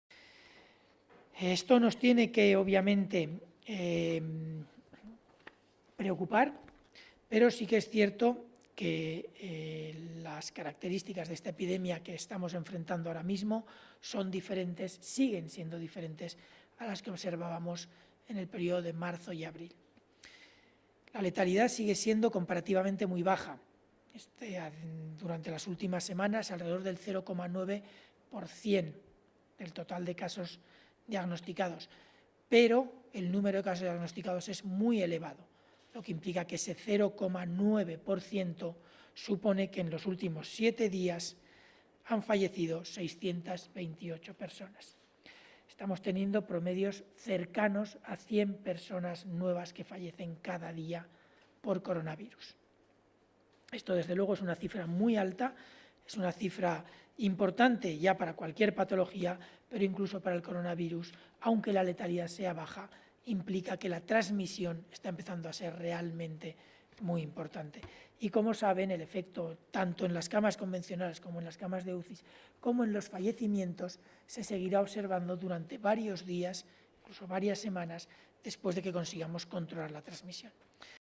Simón recalca que en la última semana han fallecido 628 personas por Covid
El director del Centro de Coordinación de Alertas y Emergencias Sanitarias (CCAES), Fernando Simón, ha recalcado que en la última semana, han fallecido 628 personas con diagnóstico de COVID-19 positivo confirmado en España, un leve descenso en comparación con los 655 notificados el viernes.

(Fuente: Ministerio Sanidad)